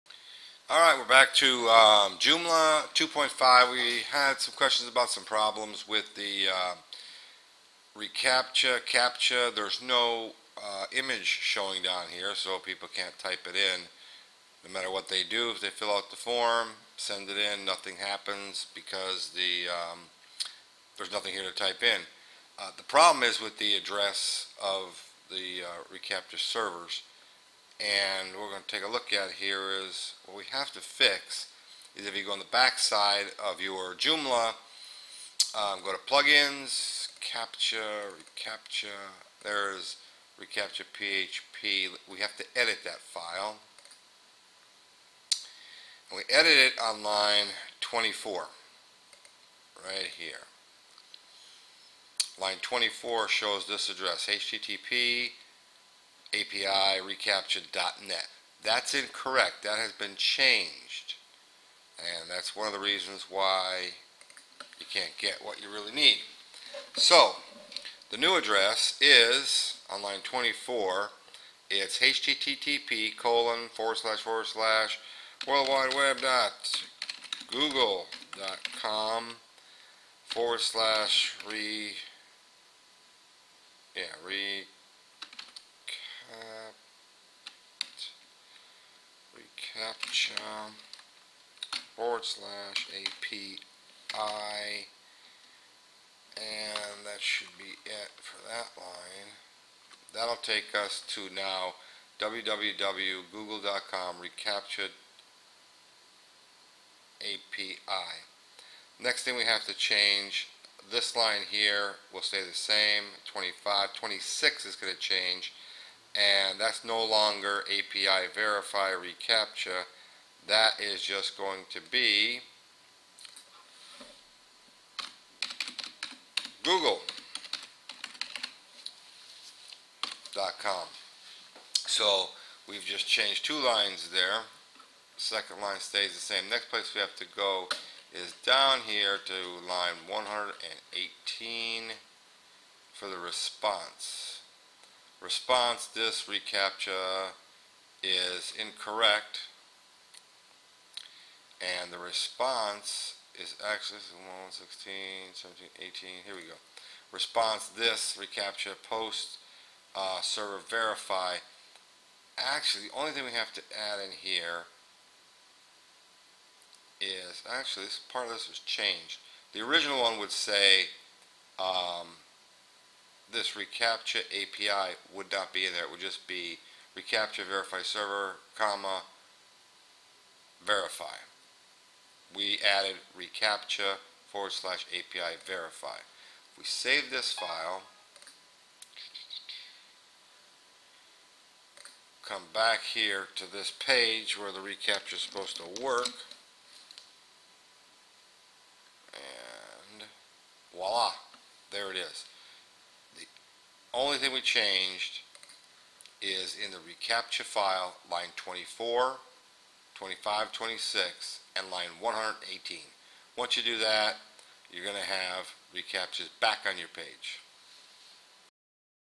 recaptcha image not displayed in Joomla 2 5
Joomla 2.5 does not display recaptcha image, modify recaptcha.php in the plugin file to fix recaptcha so that it displays on page. Problem is with the address of the recaptcha server.